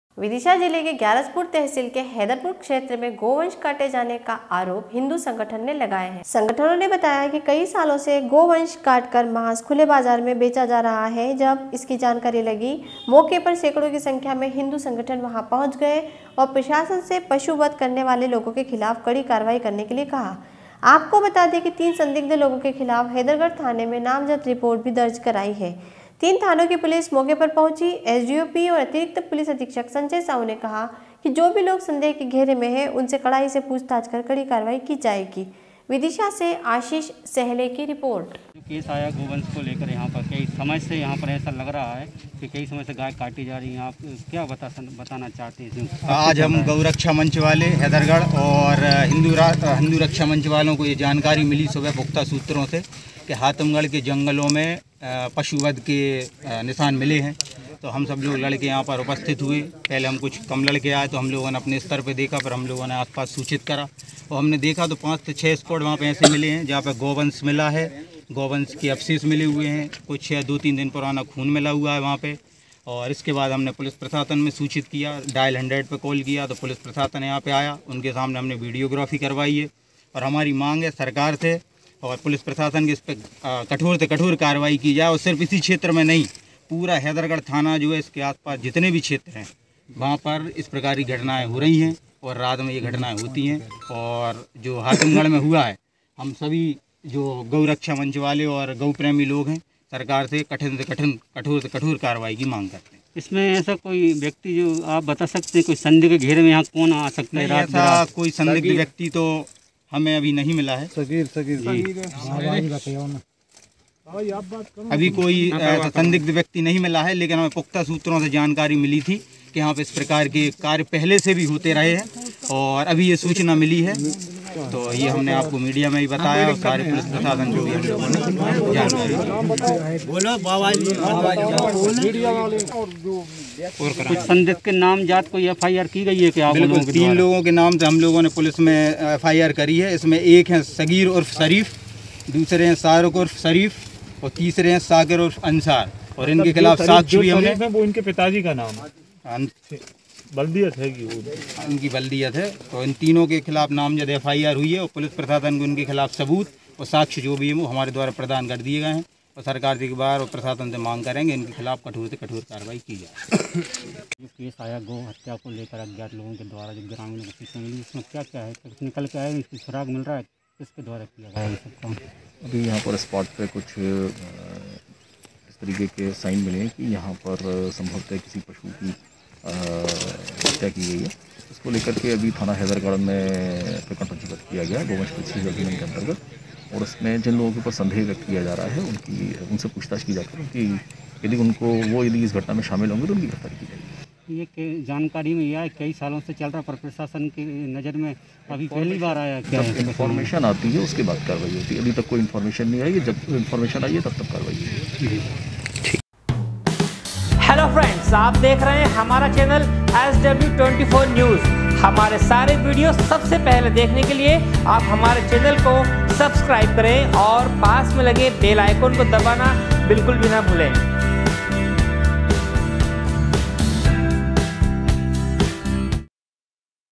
Vidisha- ग्यारसपुर तहसील के हैदरगढ़ क्षेत्र में गोवंष काटे जाने का आरोप हिंदु संगठनों ने लगाए है।
Vidisha- SW 24 News Madhyapradesh
विदिशा जिले के ग्यारसपुर तहसील के हैदरगढ़ क्षेत्र में गोवंष काटे जाने का आरोप हिंदु संगठनों ने लगाए है। विदिशा से आशीष सेहले की रिपोर्ट